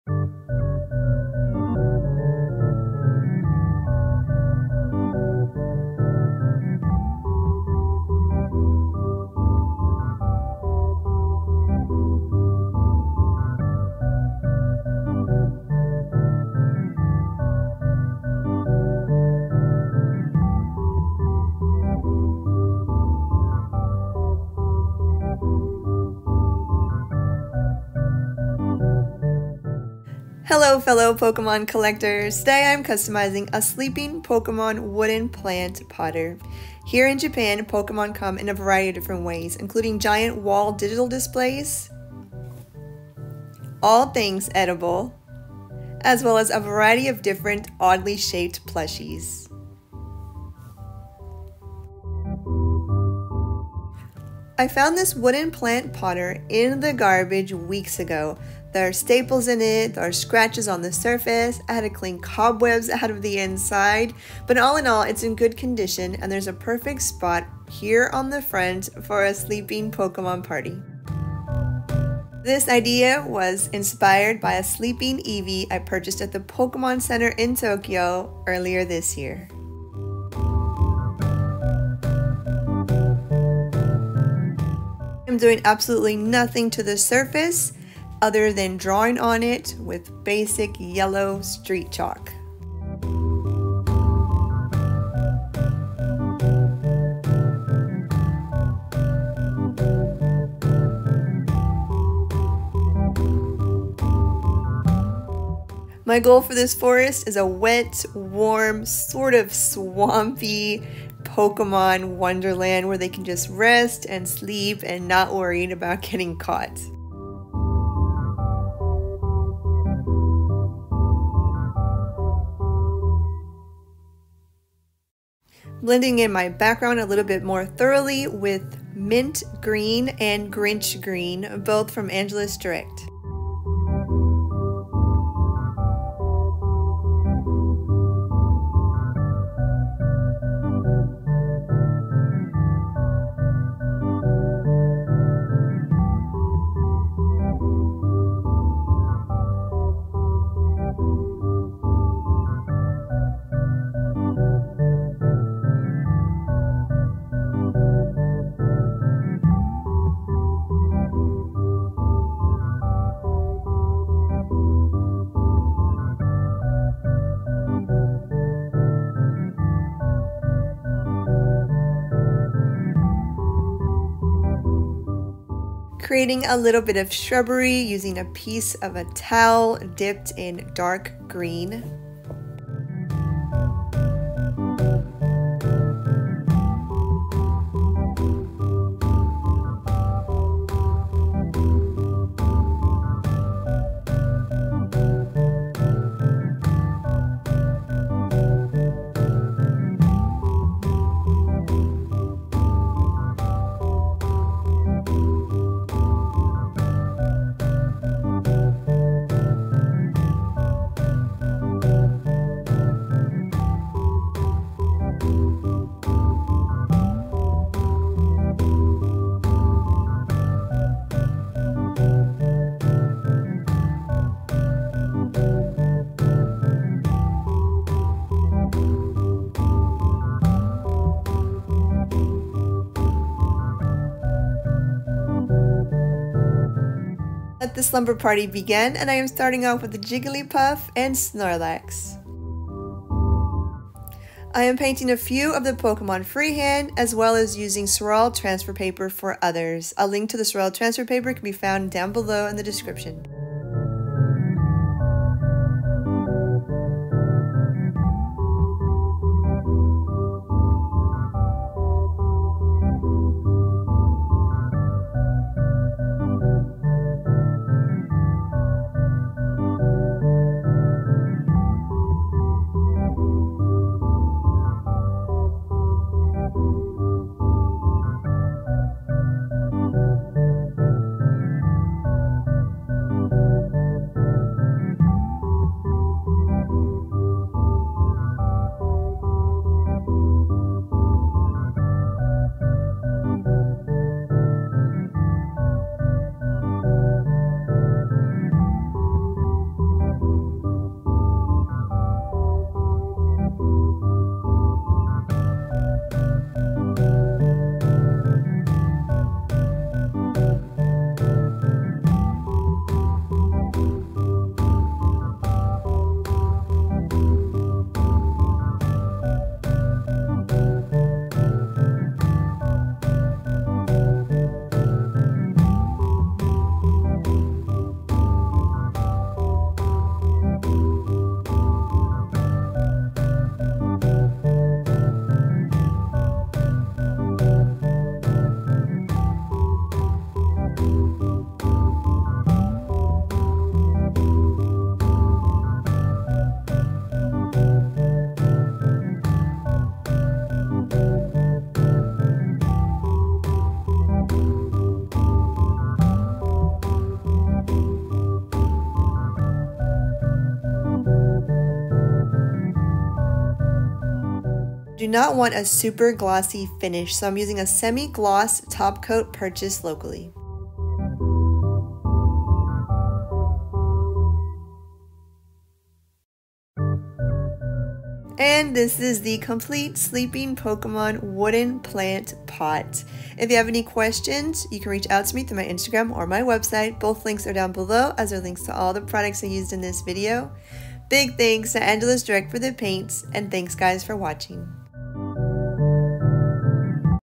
Sleeping Pokémon Wooden Plant Potter
in Japan, pokemon can be found everywhere.. from manhole covers to ramen cups to giant interactive digital wall displays. Pokémon is a way of life. Inspired by a sleeping Eevee plushie I picked up from the Pokémon Center Tokyo earlier this year I created a sleeping forest scene with a few of my favorites on a wooden planter

📎Saral Transfer Paper Blue
📎Saral Transfer Paper White
🖌Princeton Select Brushes
💦Water Repellent for Fabric